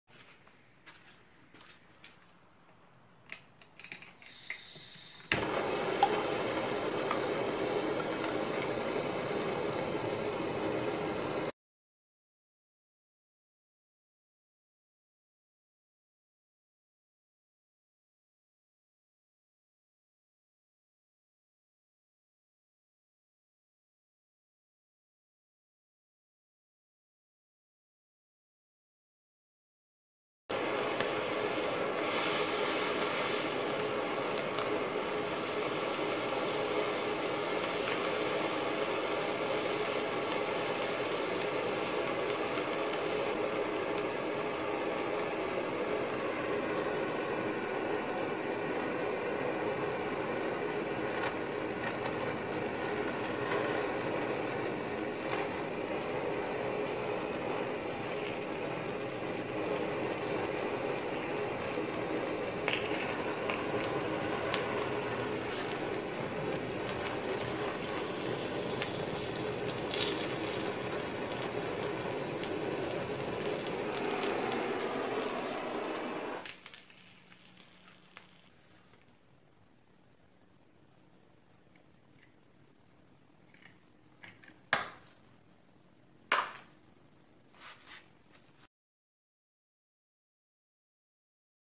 Torch And Water Bottle !!!
Can we boil water in a bottle with a torch? Let's try it.

Thanks to:
InShot
ToonApp